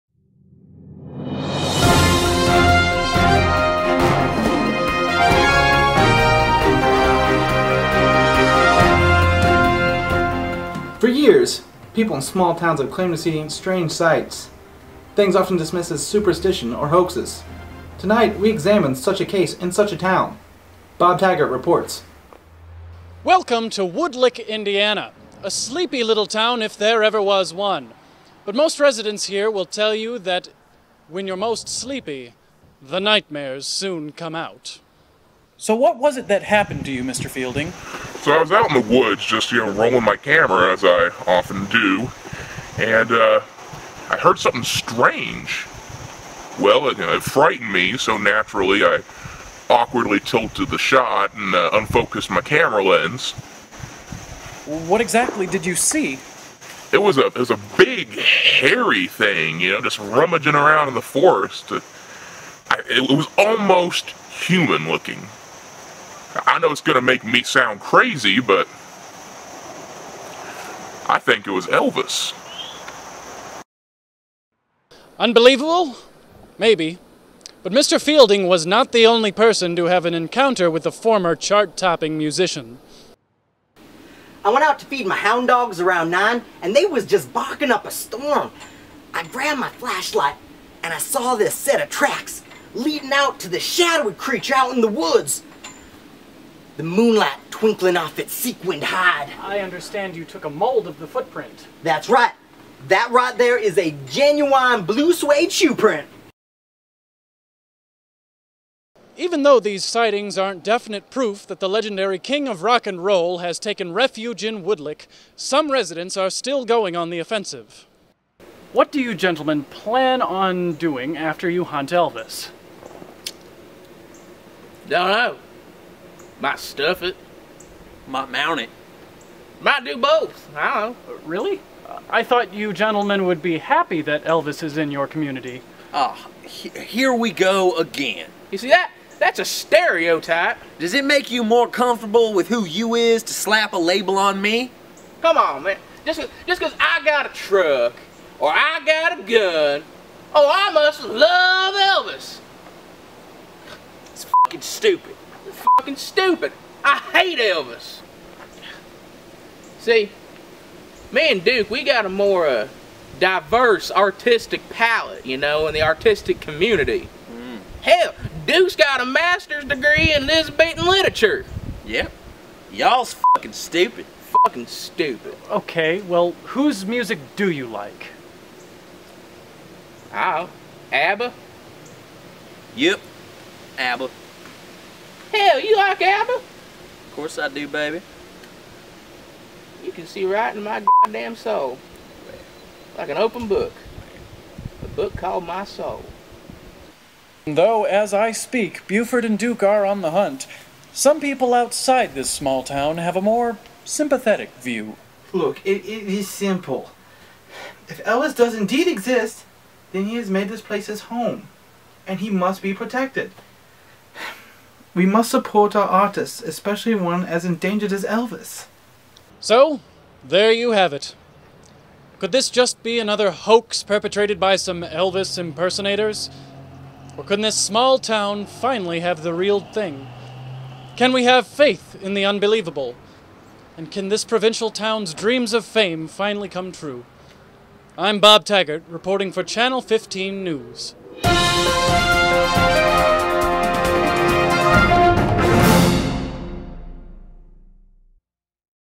Big Suede Shoes
A Channel 15 Special News Report on strange sightings in Southern Indiana.